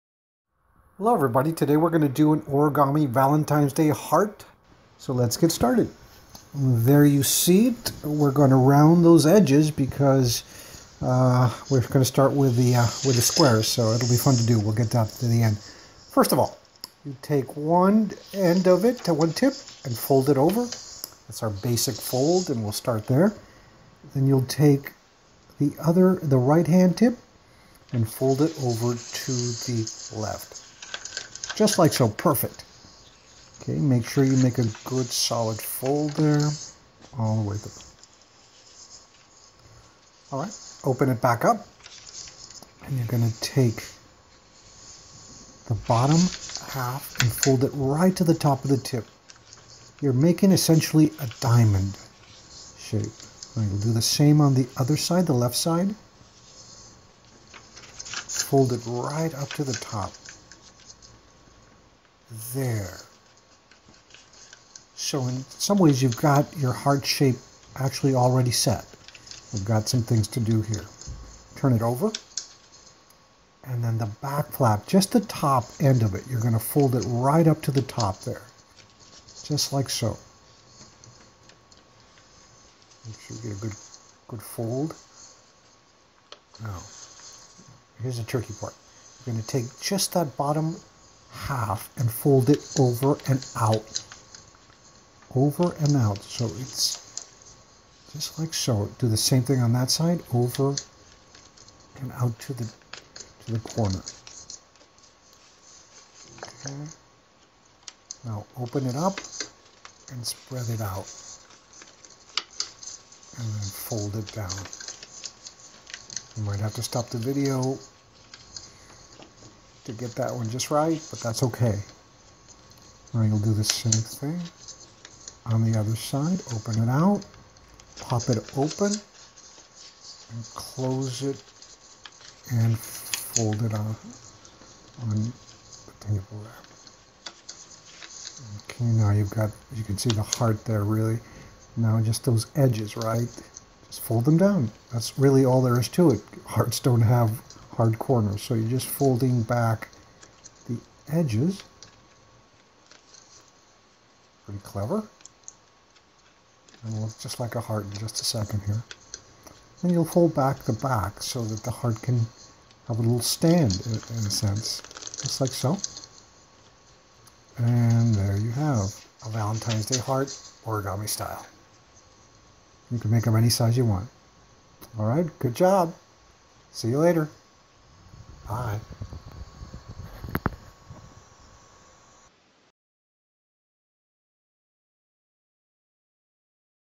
Easy Origami Tutorial: How to Make an Valentine's Day Origami Heart
Follow these easy instructions on this origami tutorial to learn how to make a cute origami heart for Valentine's Day. The key to this craft is rounding the edges to give the heart it's rounded shape.

The steps are explained in detail and even younger children should be able to follow along and make the Valentine's Day craft. Have fun!